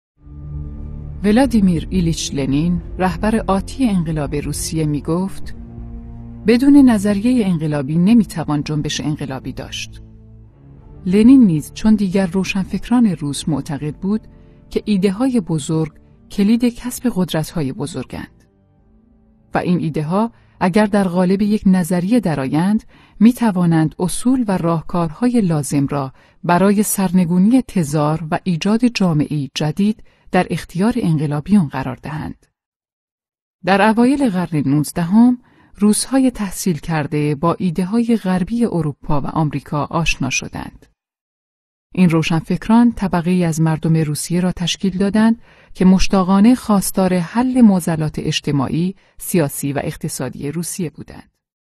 کتاب صوتی انقلاب روسیه
کتاب صوتی انقلاب روسیه اثر جان ام. دان ترجمه سهیل سمی با صدای اکرم پریمون. نسخه کامل این کتاب صوتی را می توانید از سایت یا اپلیکیشن آوانامه دانلود کنید: